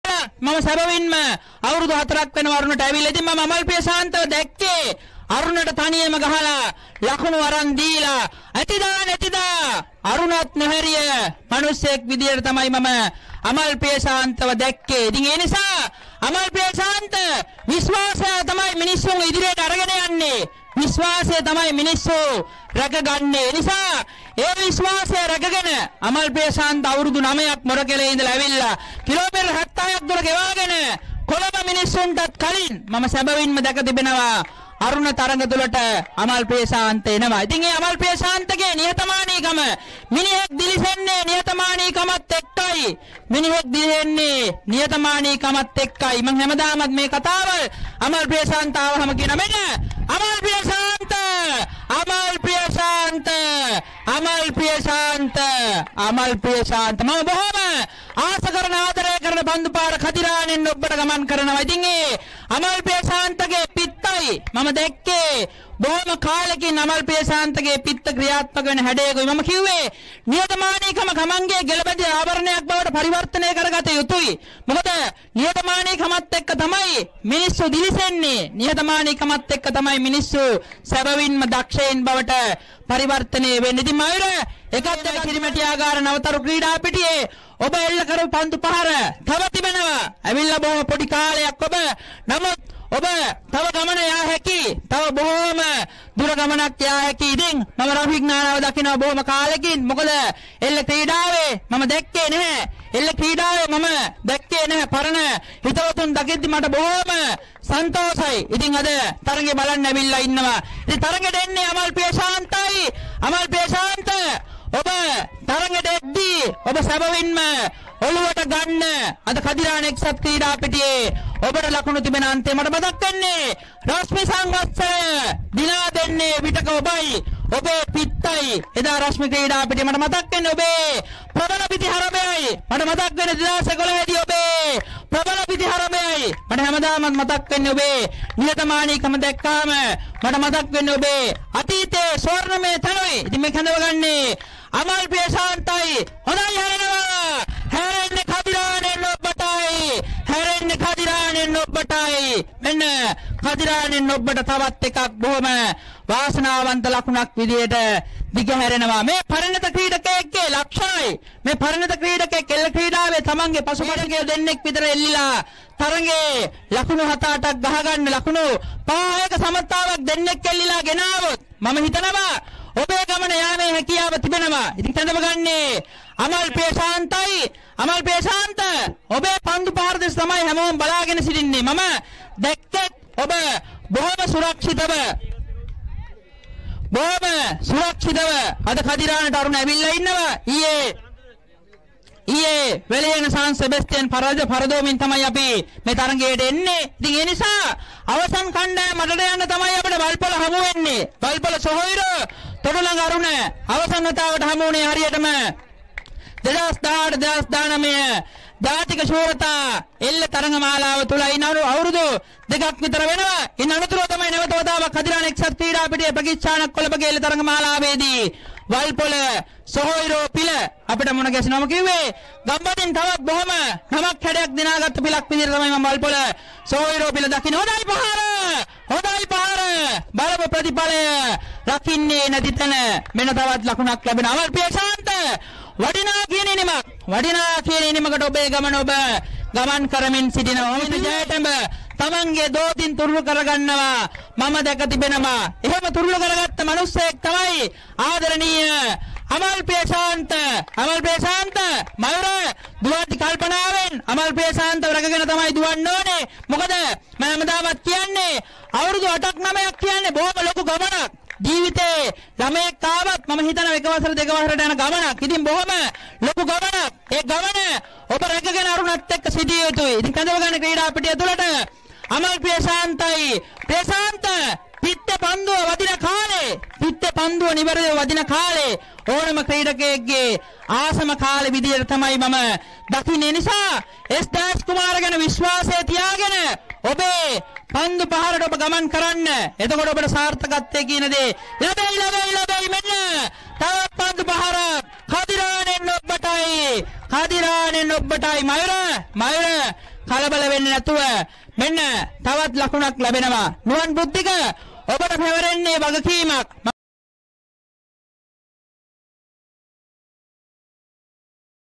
තොටළඟ අරුණ ඉනිම ගොඩගත් අමල් ප්‍රියශාන්තගේ සුපිරි පිතිකරනය
තොටළඟ අරුණ ඉනිම ගොඩගත් අමල් ප්‍රියශාන්තගේ සුපිරි පිතිකරනය,කදිරාන එක්සත් පිටියේදී
Video credit - Elle live